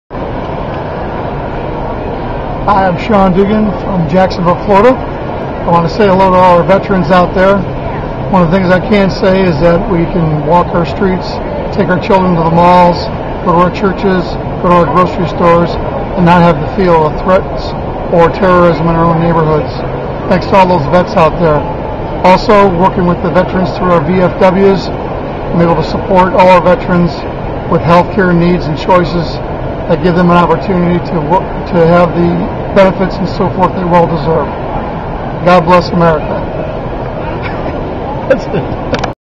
Shawn Dugan - - Thank You Troops! from the VFW Convention
Video recorded live at the VFW convention in the UAW-GM exhibit,